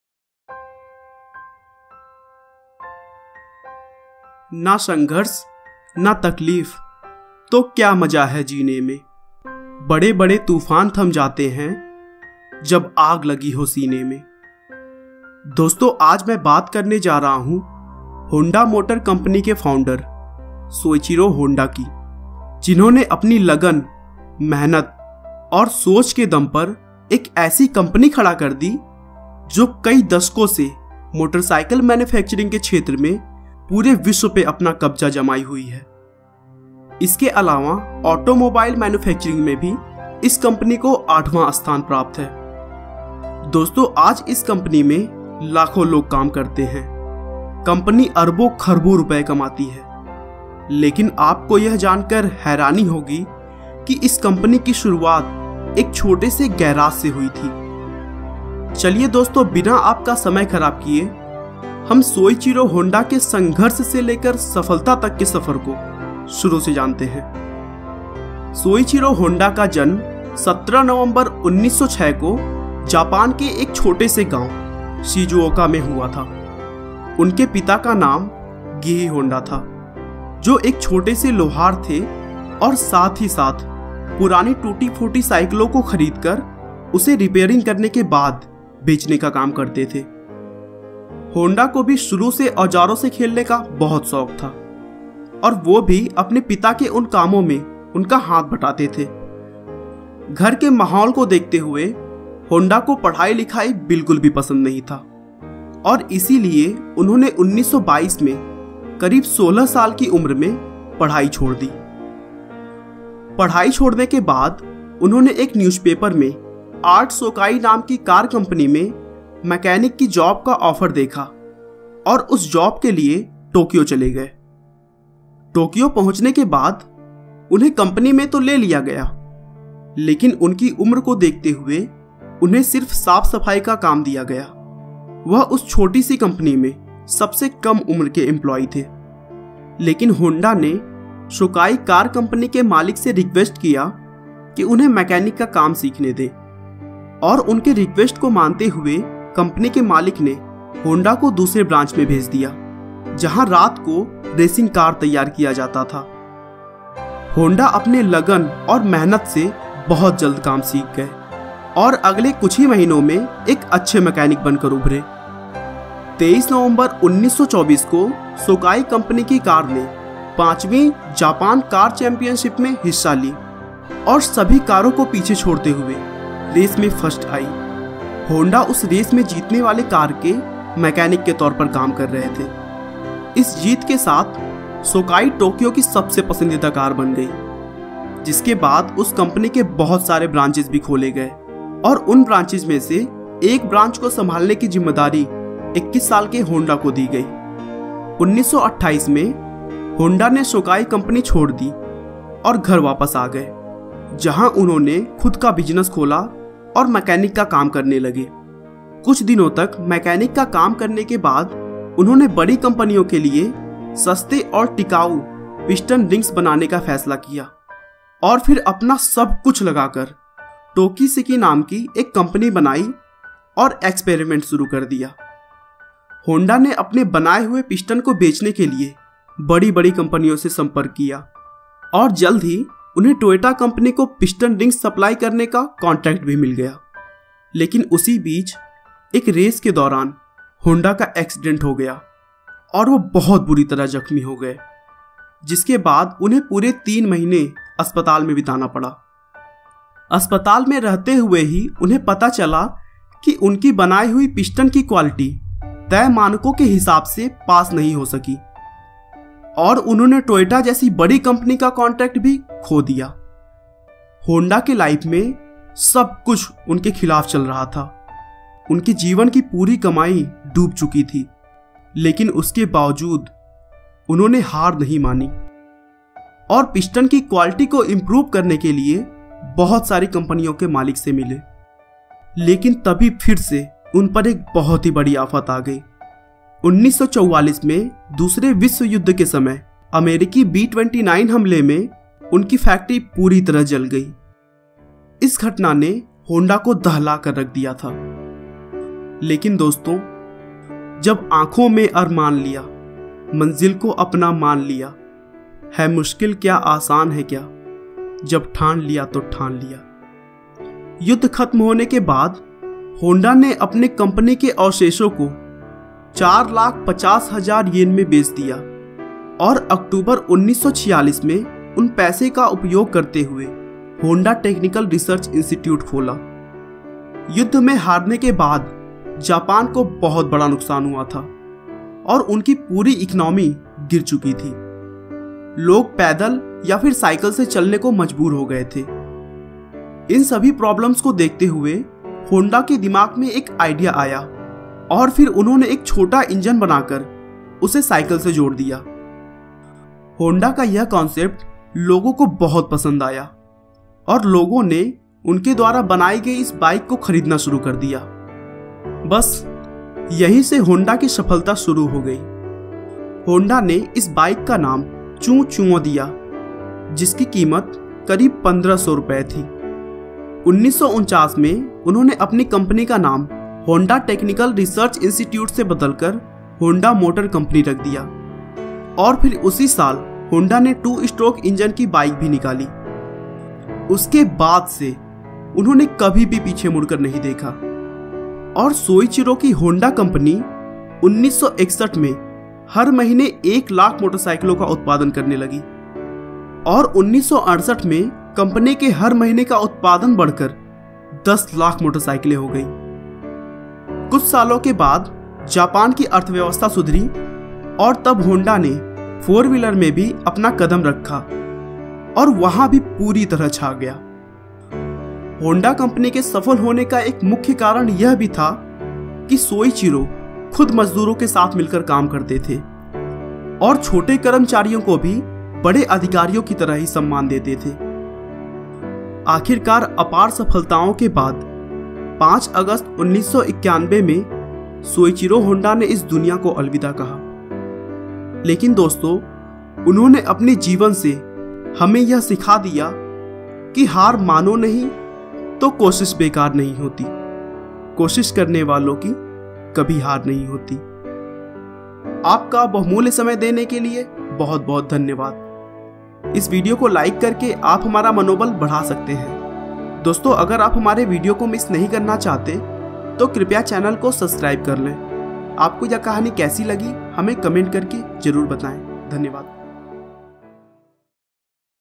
Soichiro Honda 🚗 True Story In Hindi | Honda Motor Company | Inspirational And Motivational Video
This is true story about founder of Honda Motor Company " Soichiro Honda " in Hindi.